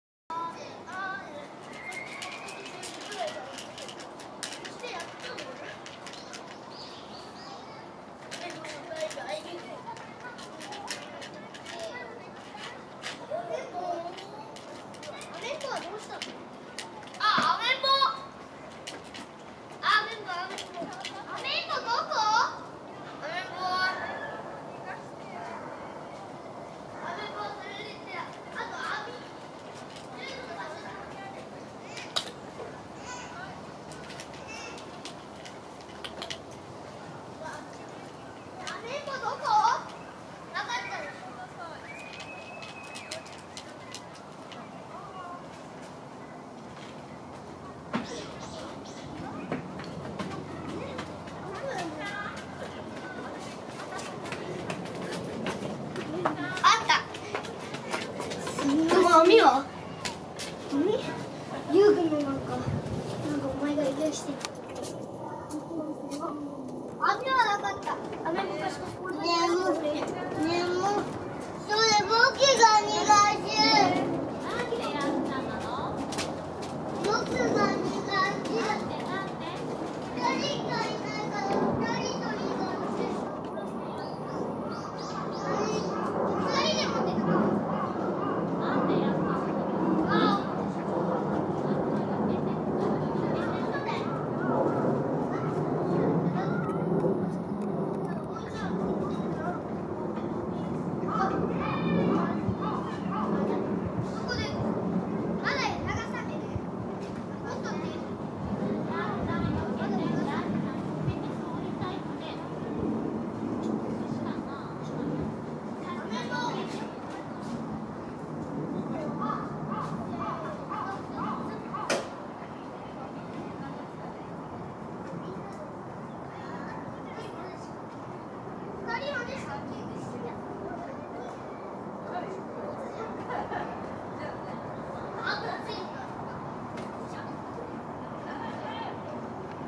出演猫、ウスとボス。
ウスは地域猫ボランティアの方が餌を食べさせていいという、お墨付きを頂いたためにわたしが餌をあげています。
ボスが急いで東屋から逃げた理由は、小学生が数人押し寄せてきて、子供が嫌いなボスは真先に逃げていきました。